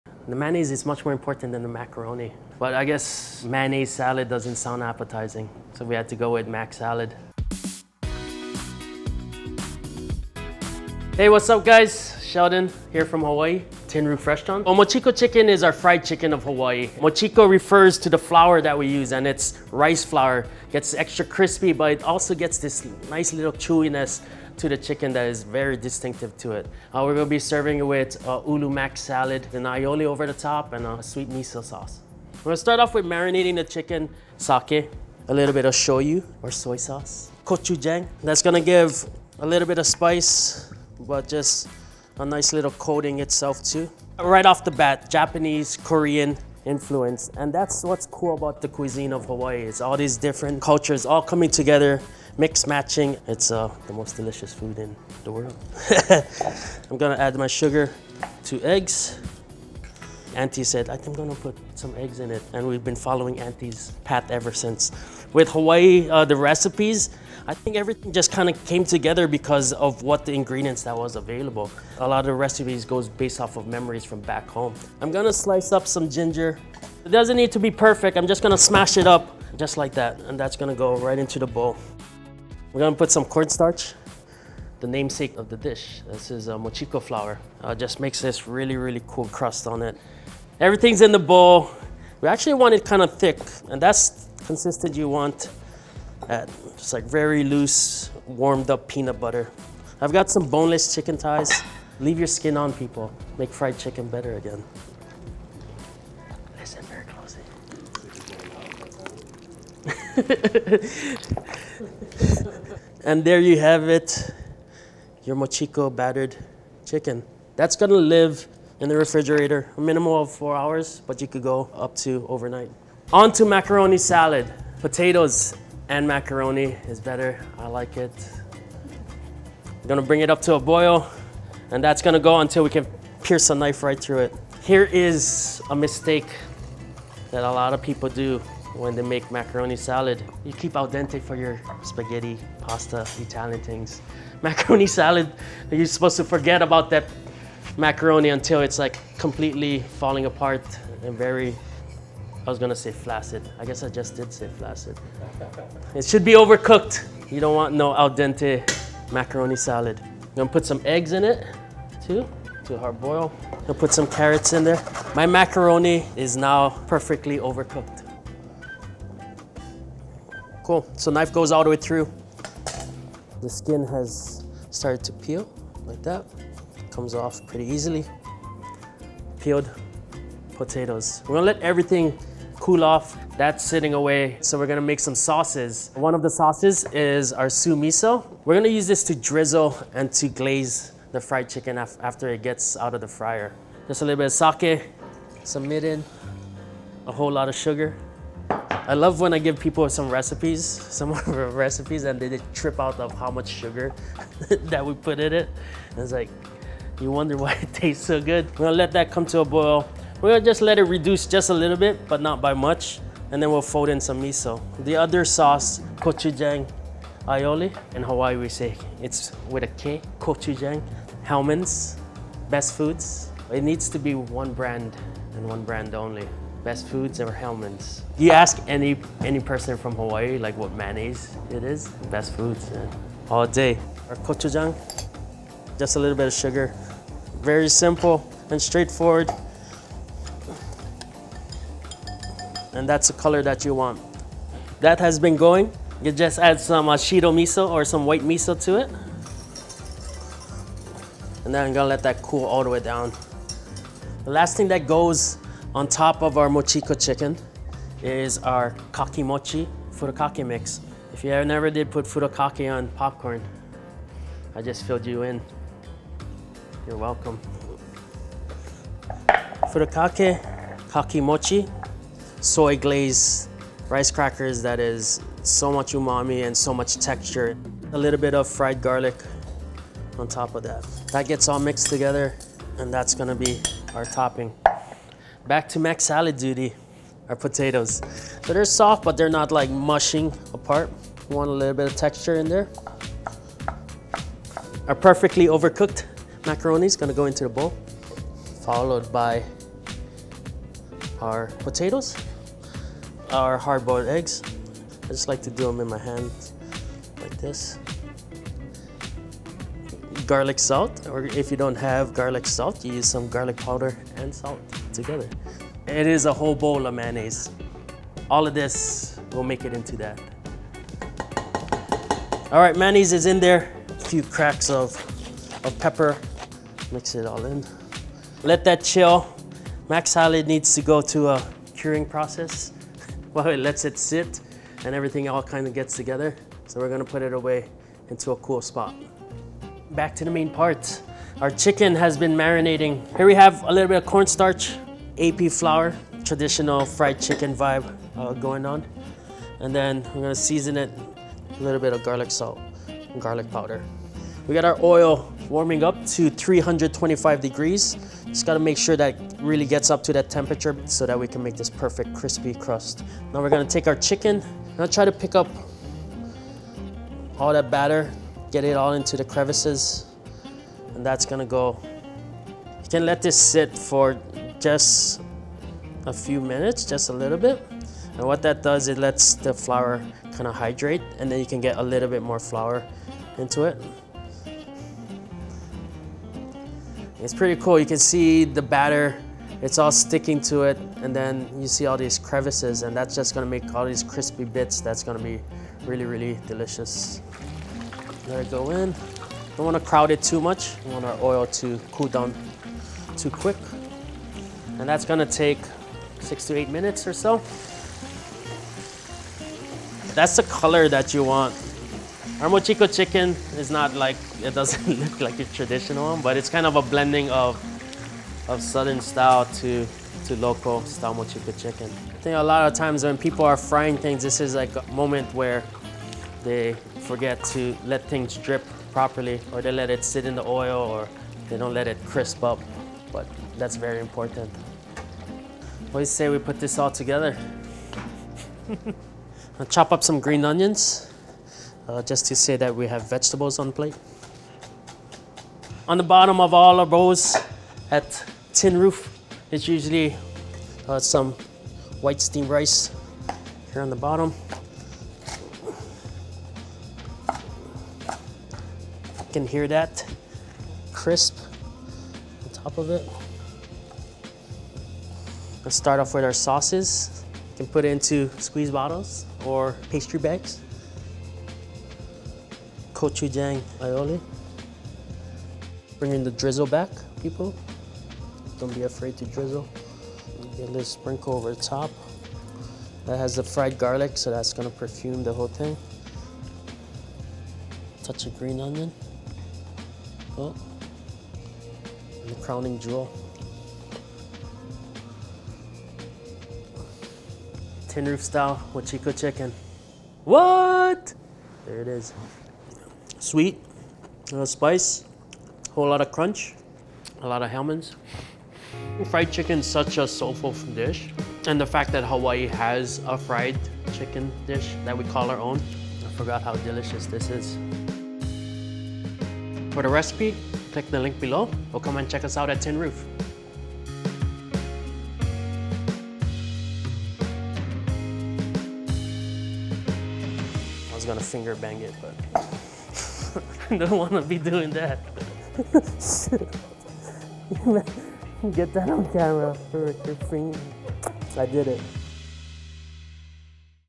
Hawaiian Fried Chicken - How To
Chef Sheldon Simeon of Tin Roof and Lineage shares a treat from his homeland of Hawaii: Mochiko Chicken. This fried chicken recipe is made with mochiko flour, a special rice flour made from sticky white rice, and is served with traditional (and mayo-heavy) Hawaiian macaroni salad.

Sheldon starts by marinating boneless chicken thighs in batter made from sake, soy sauce, ginger, garlic, gochujang, sugar, eggs, and mochiko flour. Next, he prepares the macaroni salad, which requires perfectly overcooked elbow macaroni, diced boiled potatoes, chopped hard-boiled eggs, garlic salt, grated carrots, and a whole bowl of mayonnaise. After resting it, he dredges the marinated chicken thighs in all-purpose flour and deep fries it in canola oil until golden brown. Sheldon assembles the dish in a bowl with white rice, sliced fried chicken, and drizzles two sauces on top: su-miso and gochujang aioli. The bowl is topped with furikake (dry Japanese seasoning), kakimochi (soy glazed rice crackers), fried garlic, and green onions. Finally, Sheldon tops off the dish with a scoop of macaroni salad.